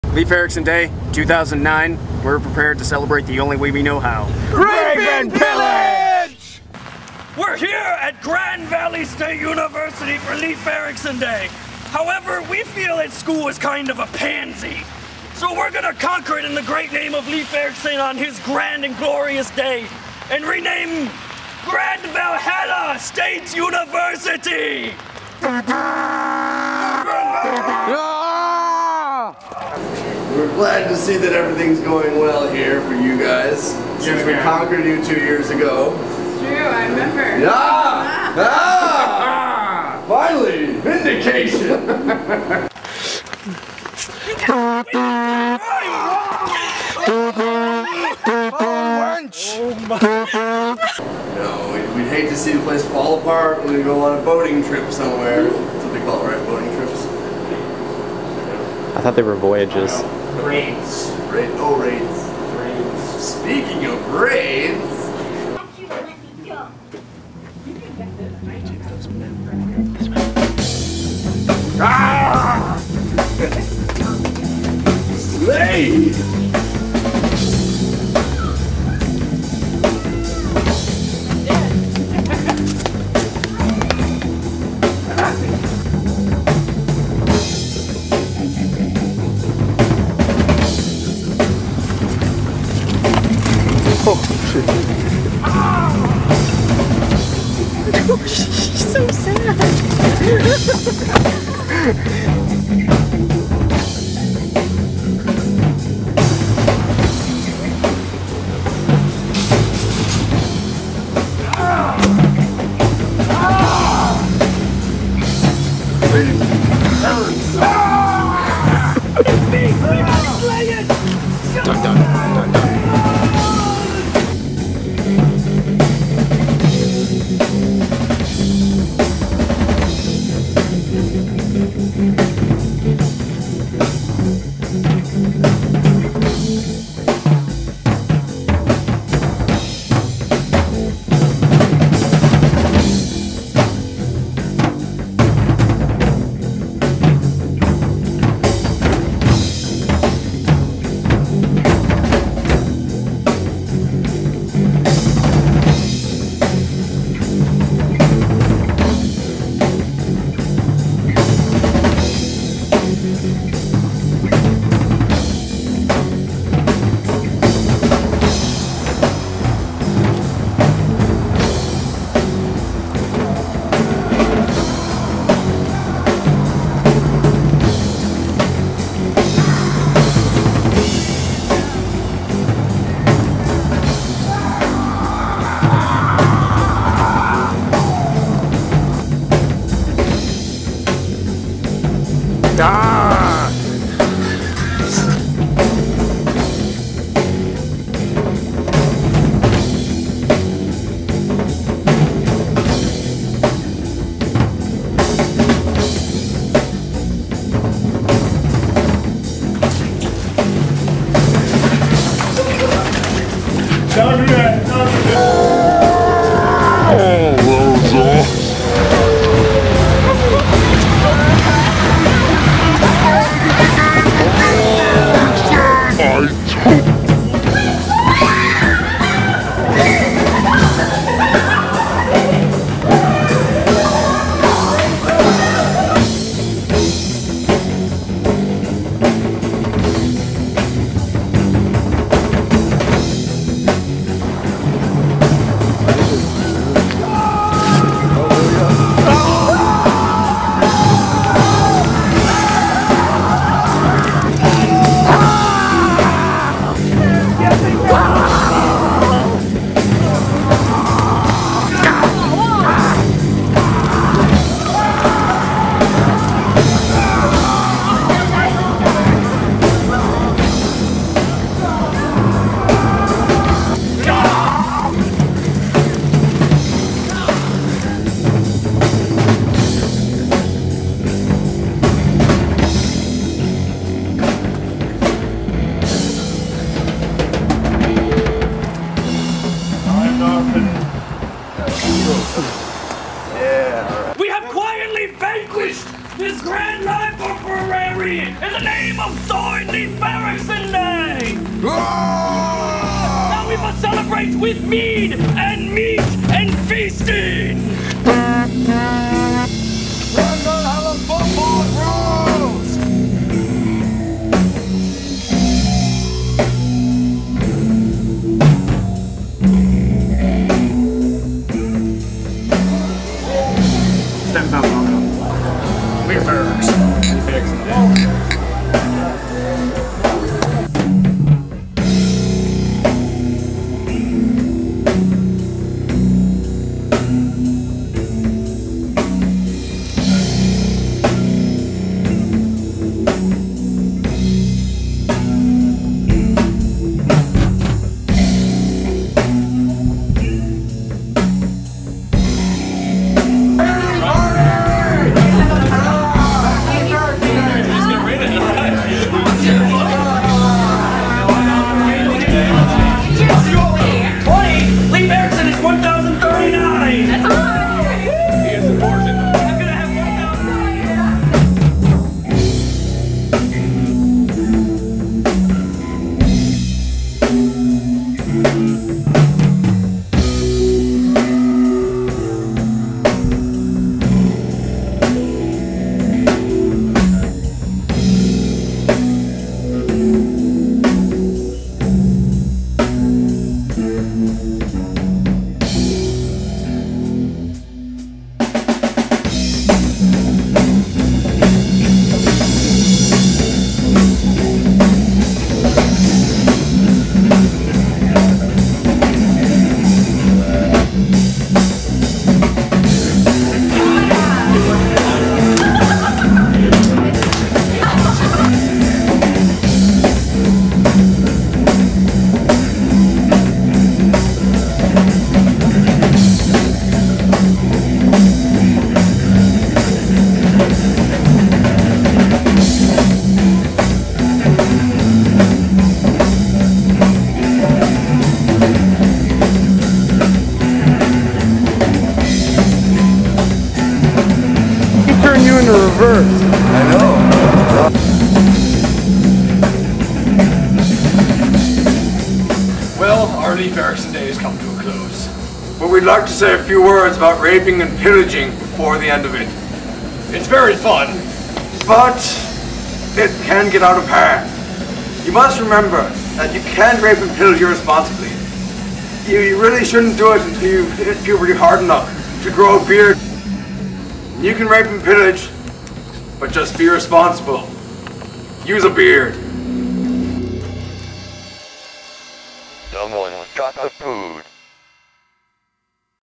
Leif Erikson Day 2009
this year, in celebration of Leif Erikson day, Mad Gear (with the help of a few new recruits) divided into two factions and conquered both sides of the state